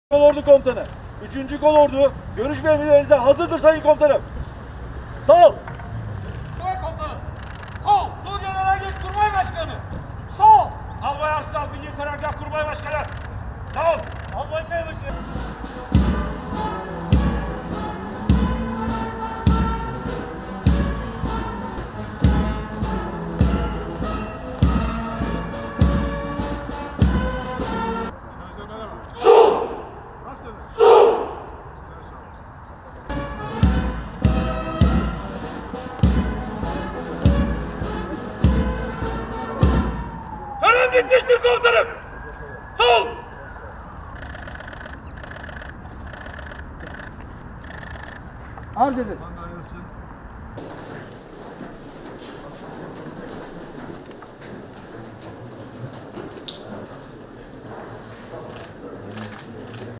Milli Savunma Bakanı Hulusi Akar, 3 Kolordu Komutanlığını ziyaret etti
Milli Savunma Bakanı Akar, beraberinde Genelkurmay Başkanı Orgeneral Yaşar Güler, Kara Kuvvetleri Komutanı Orgeneral Musa Avsever, Deniz Kuvvetleri Komutanı Oramiral Adnan Özbal ve Hava Kuvvetleri Komutanı Orgeneral Hasan Küçükakyüz ile NATO Hızlı İntikal Edebilir Kolordu-Türkiye (NRDC-T) görevini 2021’de başarıyla yerine getiren 3. Kolordu Komutanlığına bir kutlama ziyaretinde bulundu.

🗞AA